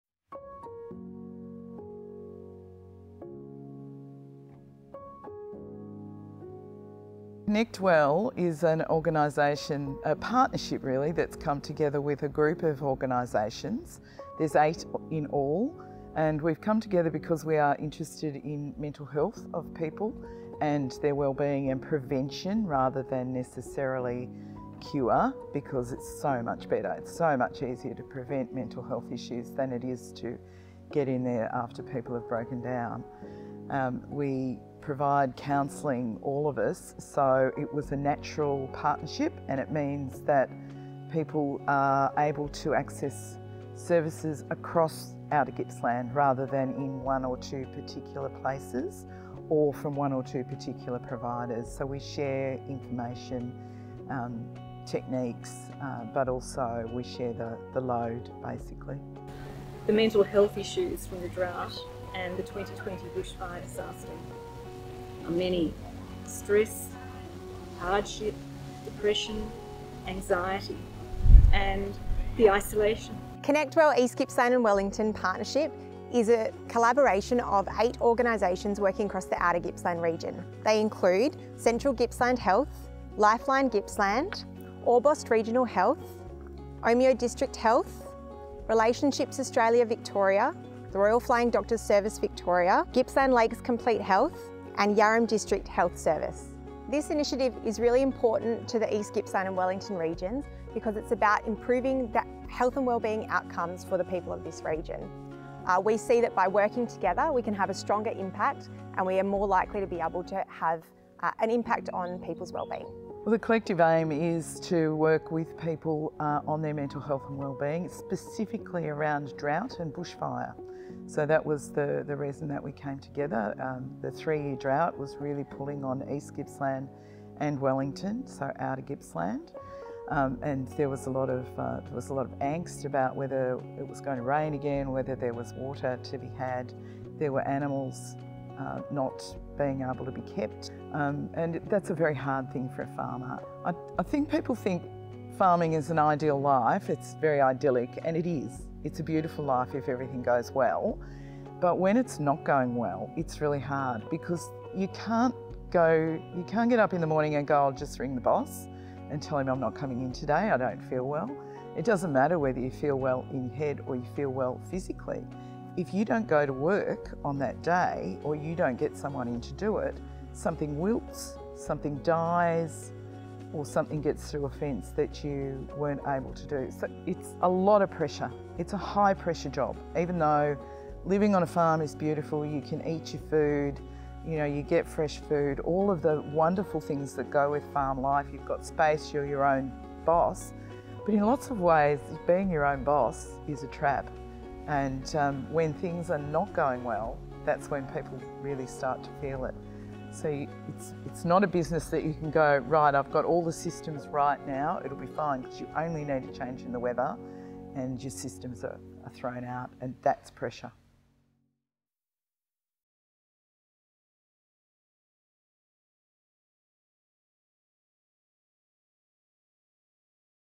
Introducing... Connect Well
Key organisations who provide community based mental health and wellbeing support in Wellington and East Gippsland Shire regions have come together to form a partnership that is known as “Connect Well East Gippsland and Wellington” (formerly the “Outer Gippsland Area Drought/Bushfire Mental Health and Wellbeing Partnership”).

This video was filmed at the launch of 2021 "Lay of the Land: Tales of strength and resilience from our dairy farmers" calendar, which is an initiative supported by Connect Well.